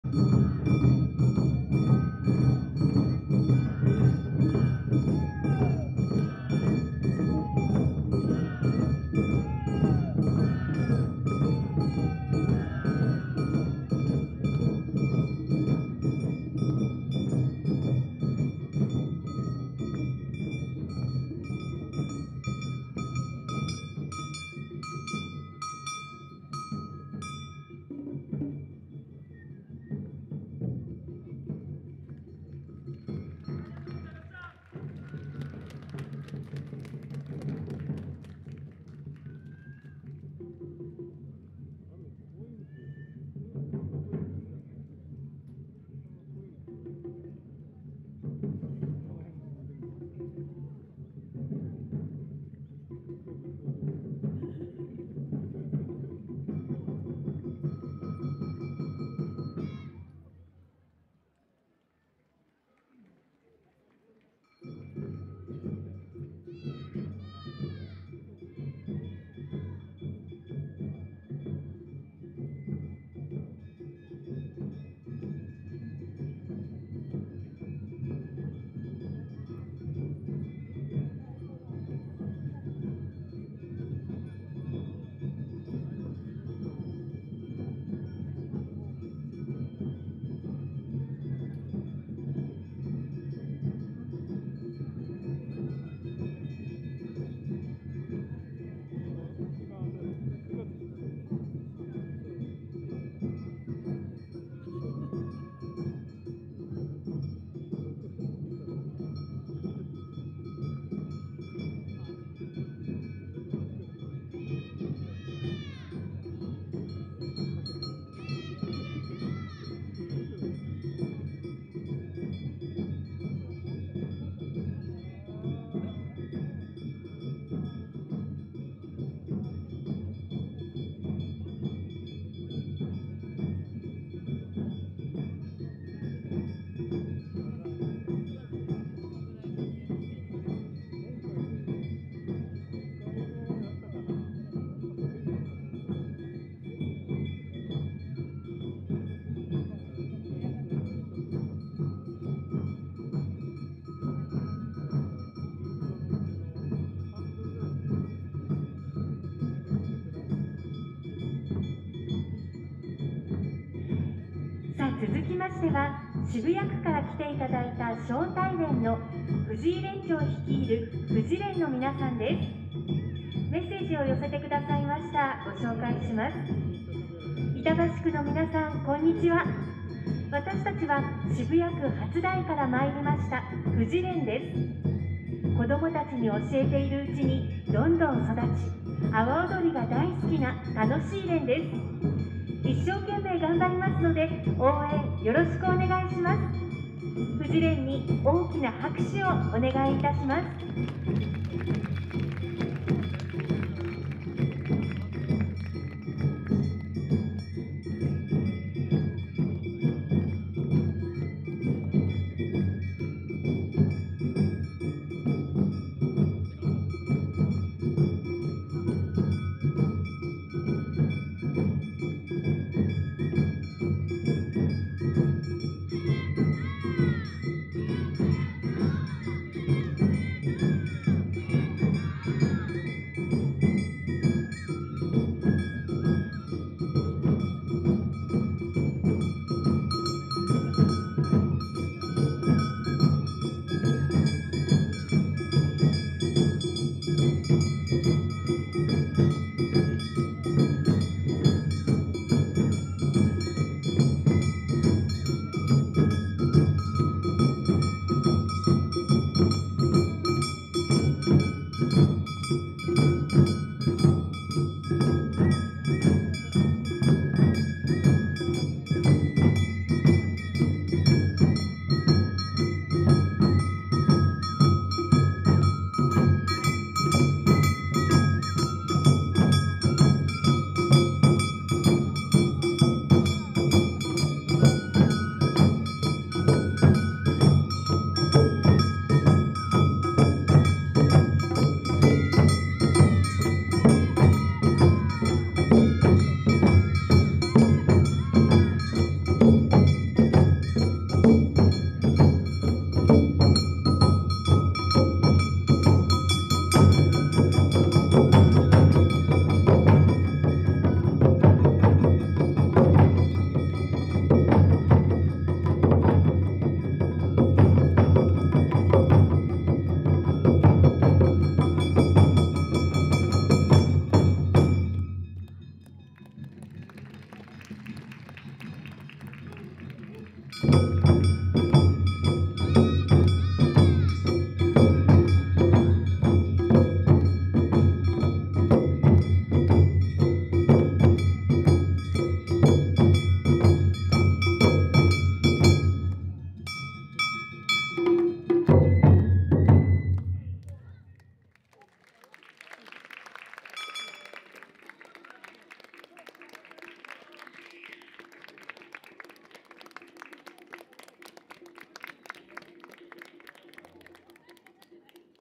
藤連 流し踊り【4K】／楽しく踊ろう！阿波踊り大会（おまつりひろば/第一演舞場）／第52回板橋区民まつり2023
2023/10/21（令和5年10月21日）
【主催】板橋区・板橋区観光協会